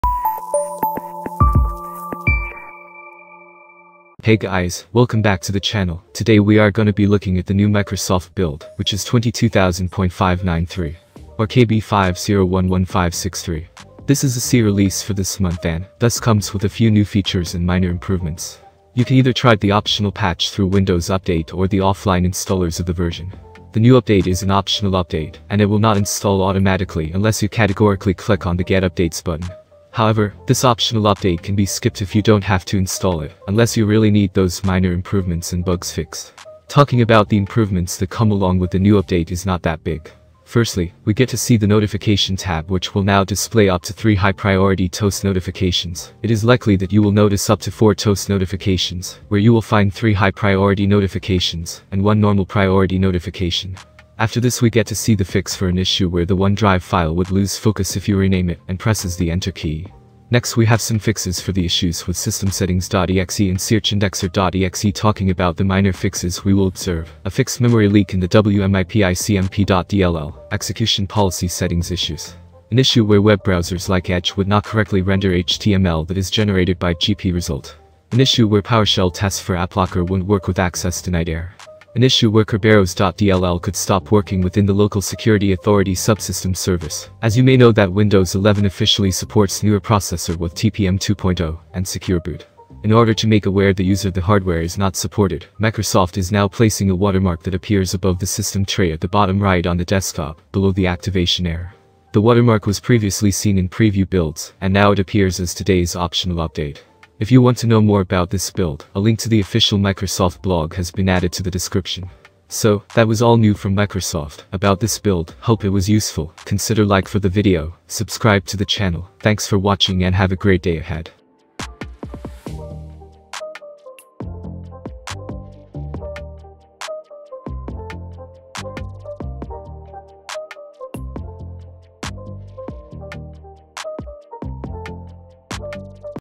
Windows 11 Build 22000.593: Fixed Memory Leaks, Lower Boot Time, Crash Fixes and More
CHAPTERS